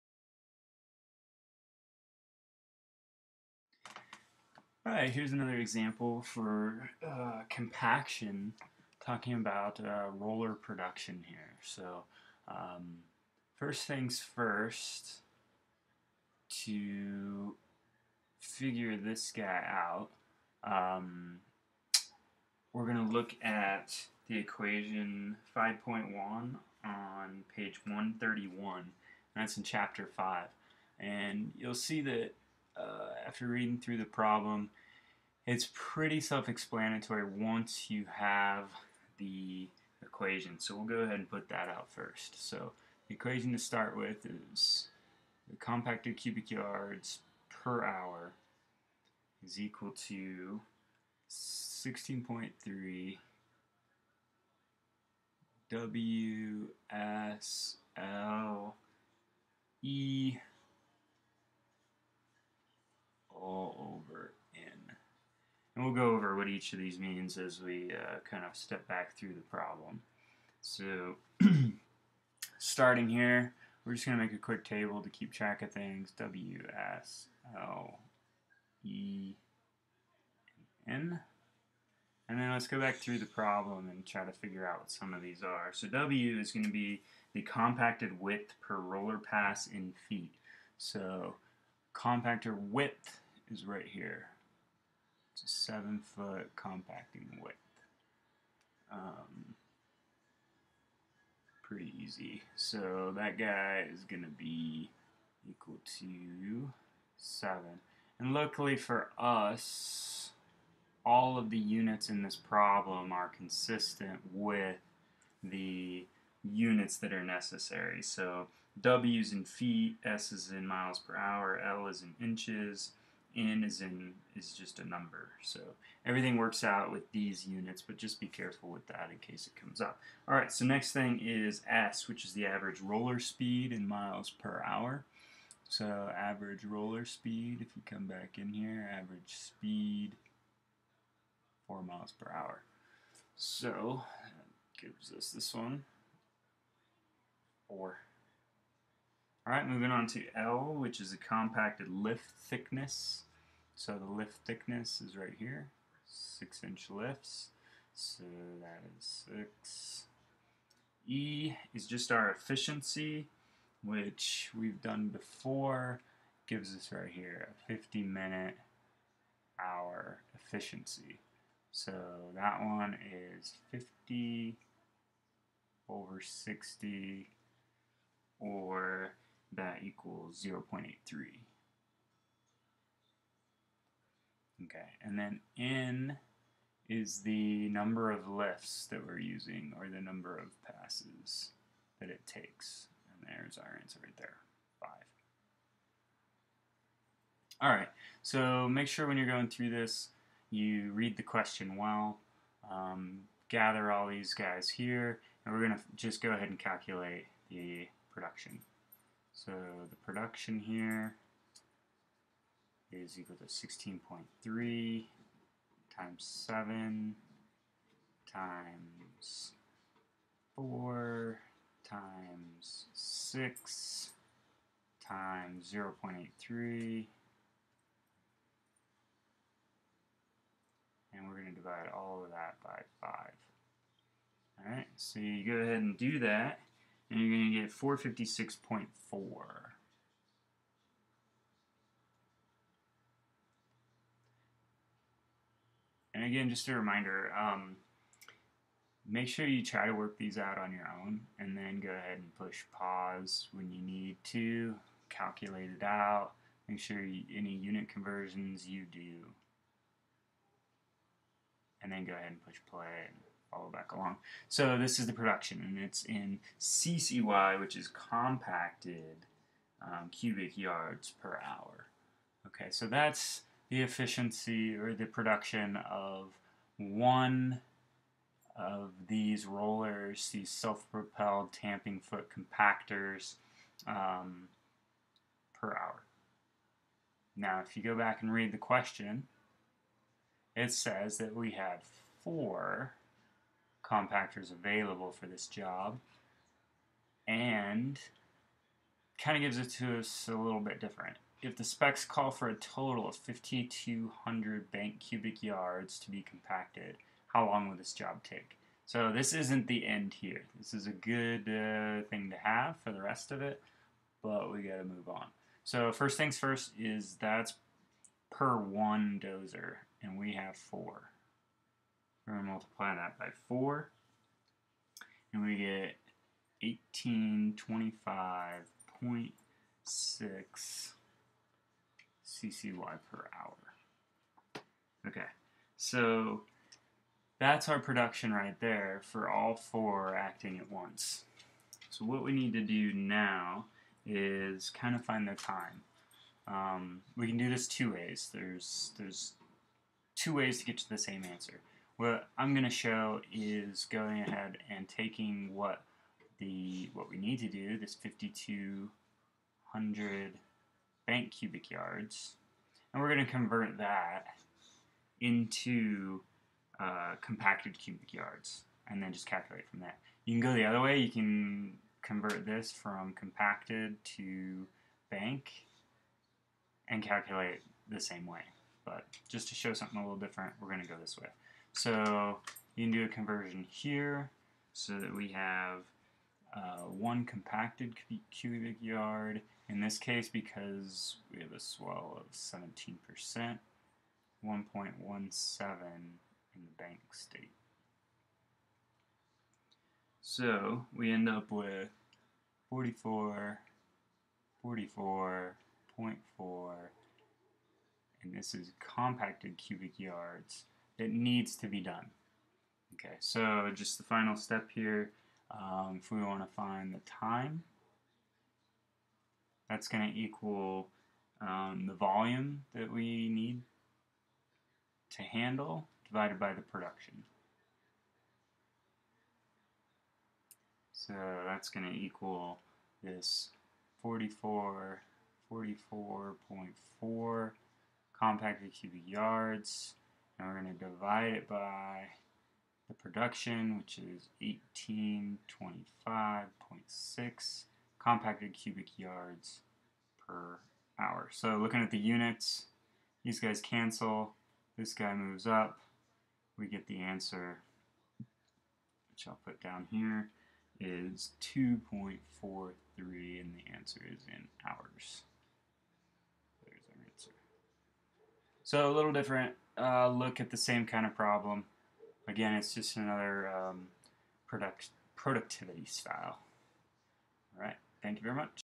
Compaction - Roller Production Example
Example problem calculating the production of compaction by a roller.
**NOTE: Annotation at 6:30 describes that the problem should technically call the conversion factor from BCY to CCY a shrinkage factor.  All calculations remain the same.